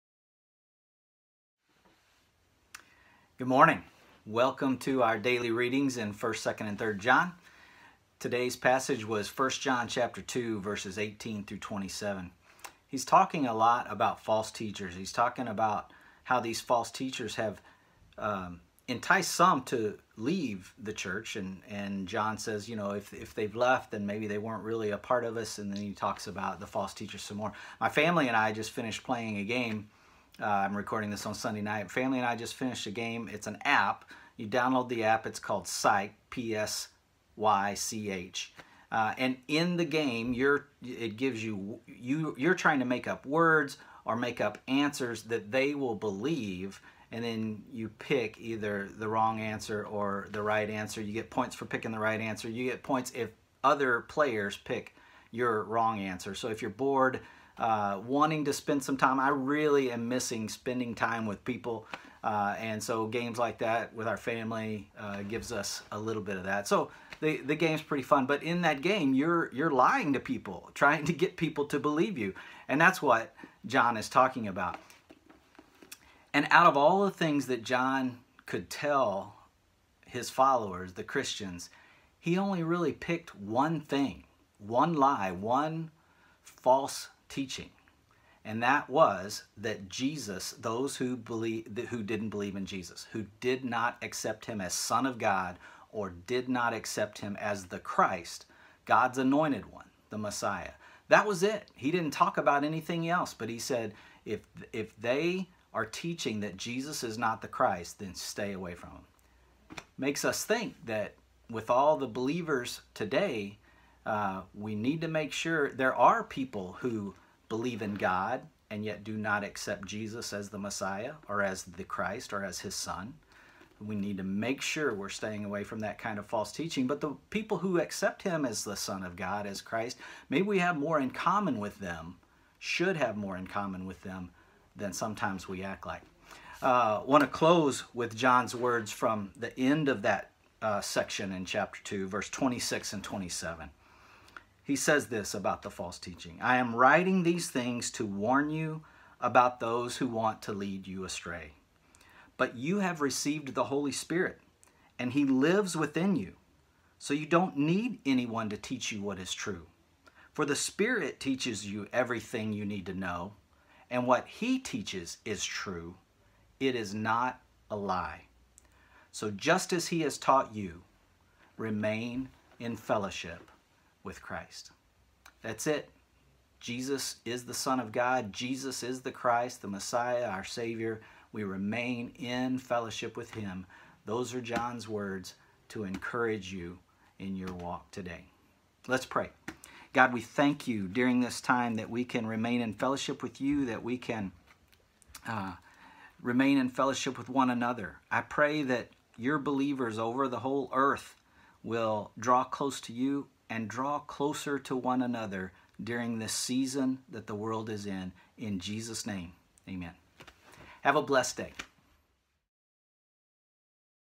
Woodbury Daily - 1 John 2:18-27
Join us as we continue our studies of 1st, 2nd, and 3rd John!